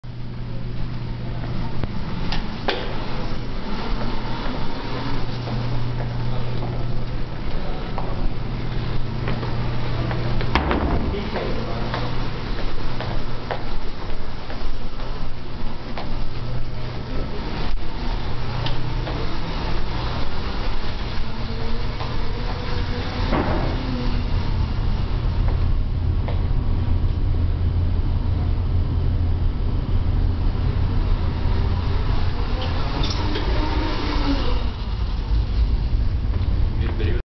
Hangar Mate Top Speed Test
The Hangar Mate top speed is estimated at 10mph.  Driving the Hangar Mate feels (and looks) similar to Mario Kart.